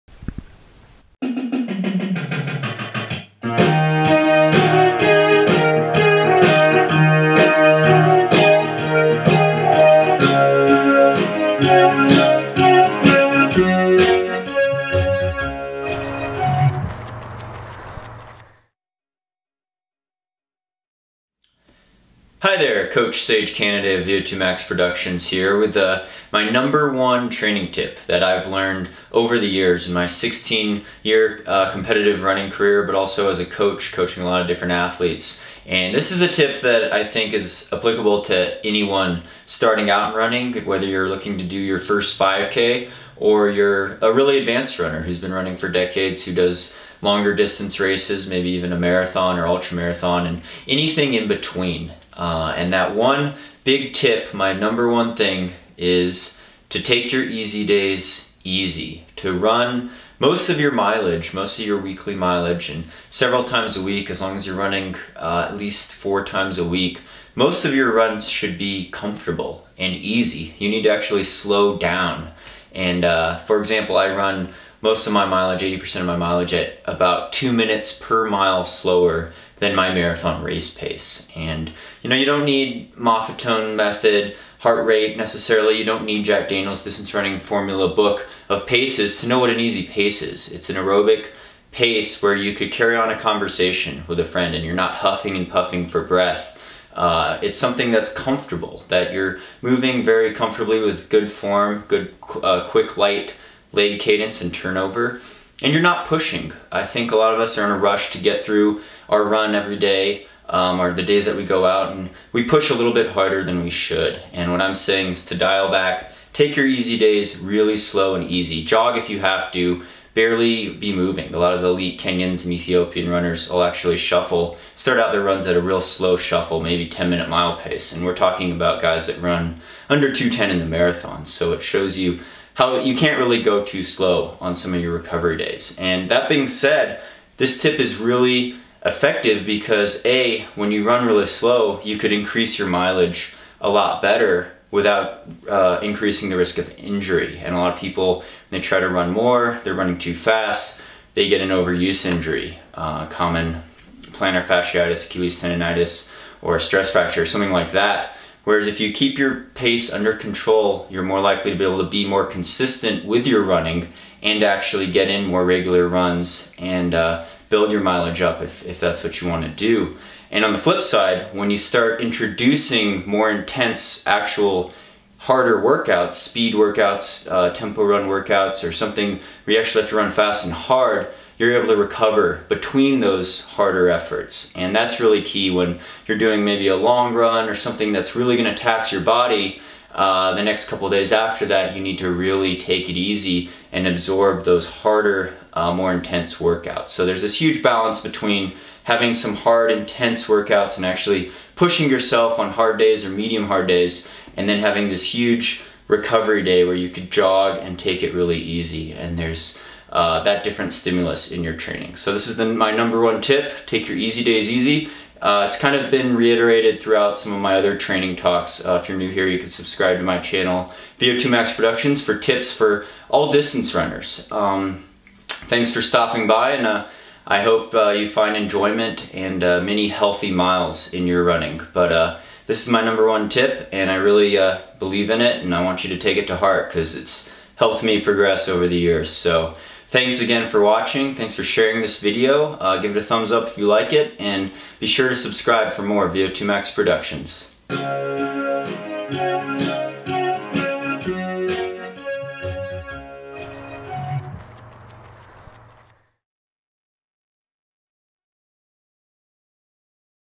BEST TIP FOR ALL RUNNERS: THE SECRET TO RUNNING FAST AND STRONG | Sage Canaday
How to run faster and further: My #1 Tip for long-term improvement in the sport of distance running.